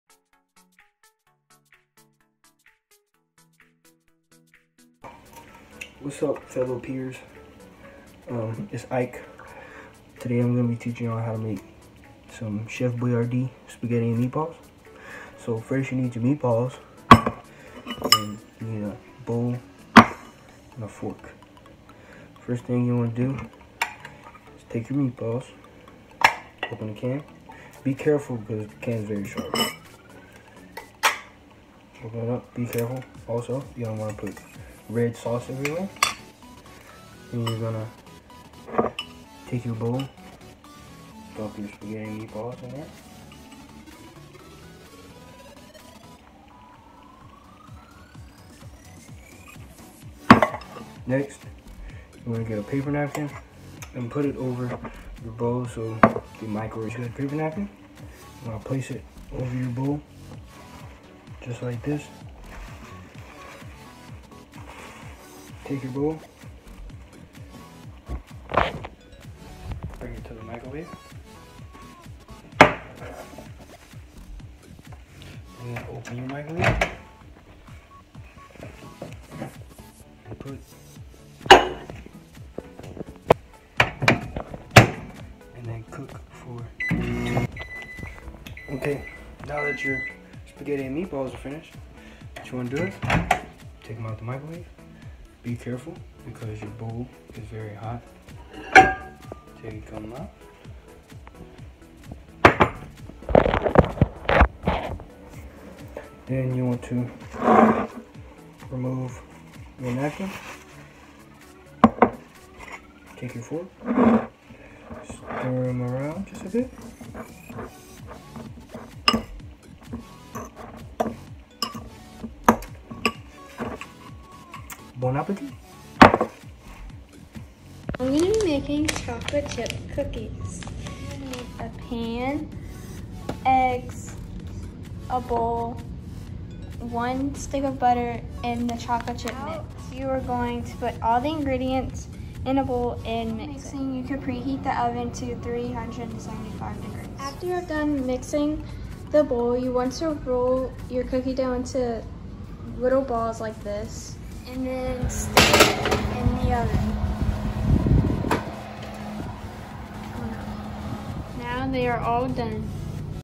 Dinner in the Den 1.27 | Salty and Sweet Quick Snacks
In today's "Dinner in the Den," we get two treats.  Senior I. Levens shows us how to warm up a quick after-school snack and M. Broussard bakes some delicious cookies.